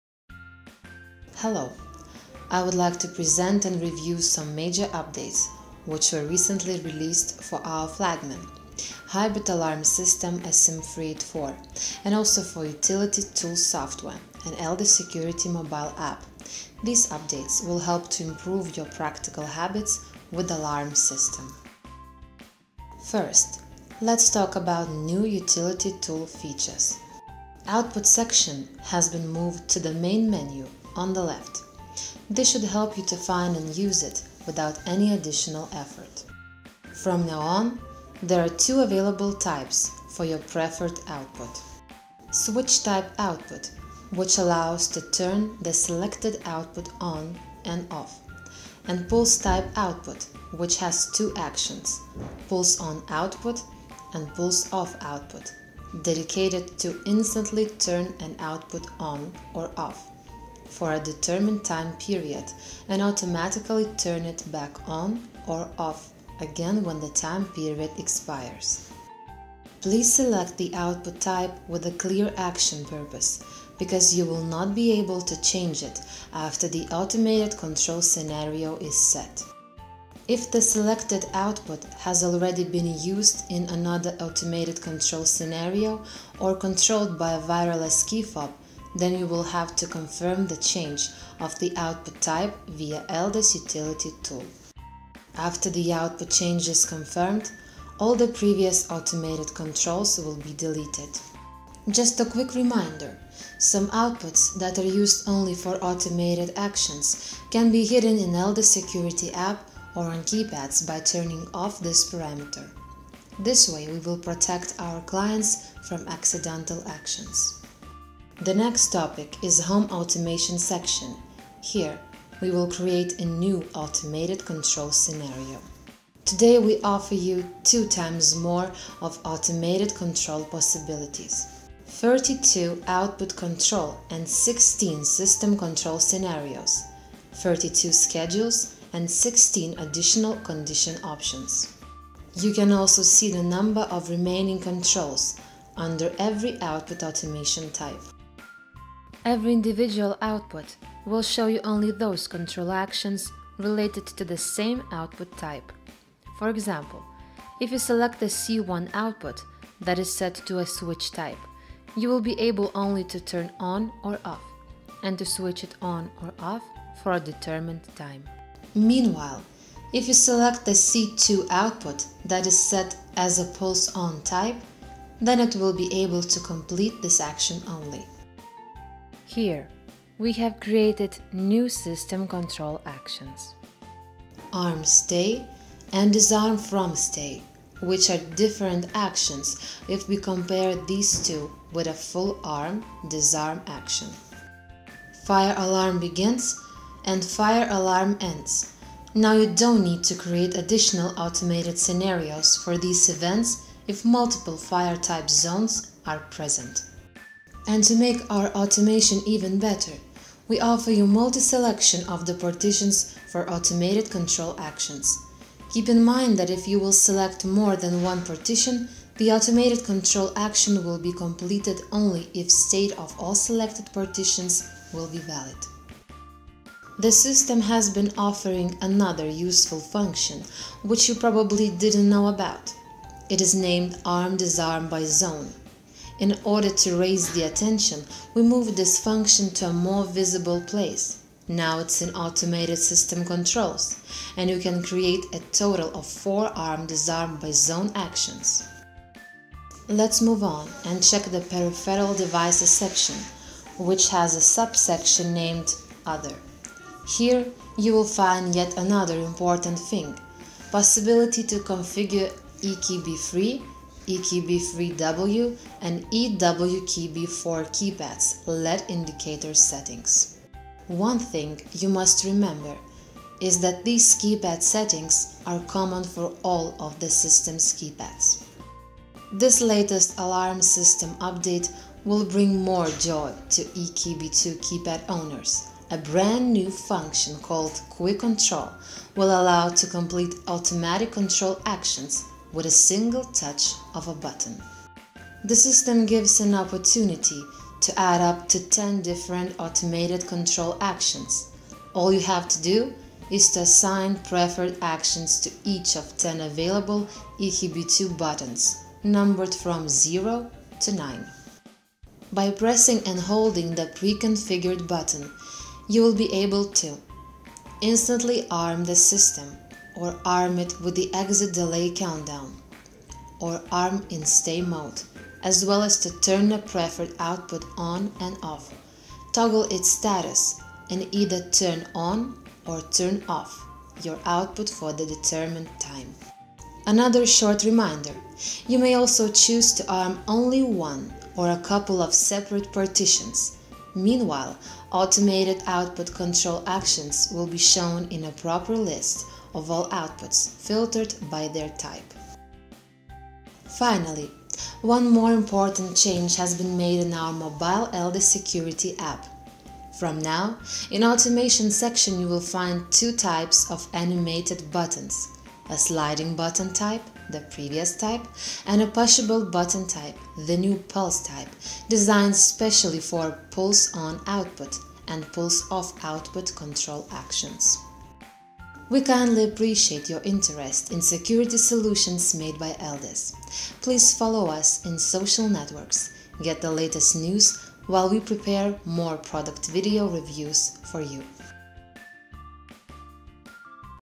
ESIM384 firmware update for your Smart Home control via Eldes Utility and ELDES Security app.
Home automation has never been so easy before. Enjoy your smart home control via ELDES Security App and Eldes Utility software on ESIM 384. Watch the video and learn how to set-up your client's needs.

Smart Home. 1-2-3 and you have it!